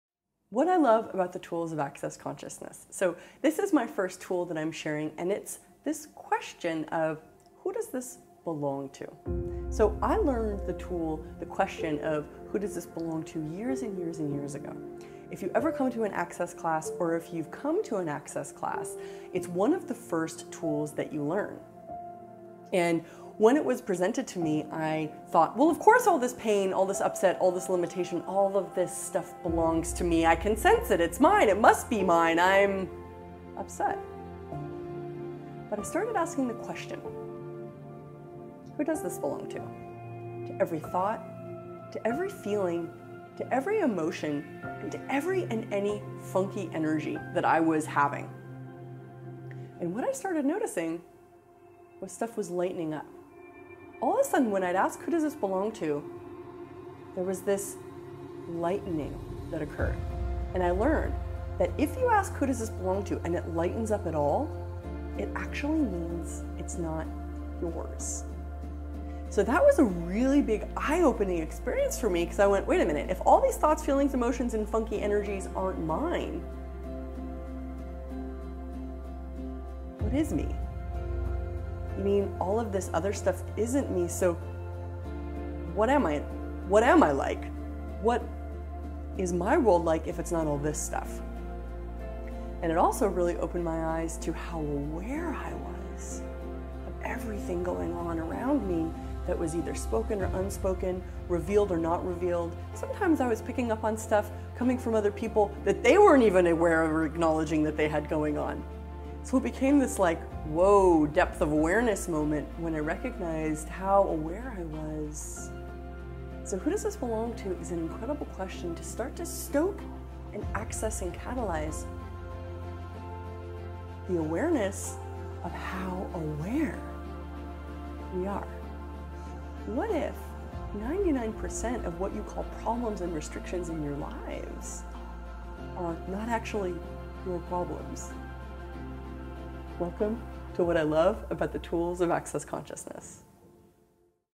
Who does this belong to? | What I Love About the Tools of Access Consciousness!!
Tool #1 - who does this belong to?

💕 What I love about the tools of AccessConsciousness !!!

There is so much i love about the tools of access the list goes on and on!

The tools have changed my life so dynamically for the better and lighter I can’t wait for share my love for this amazing resource with all of you.

Have fun, check these videos out and see what you can change in your life with these simple and profoundly dynamic tools.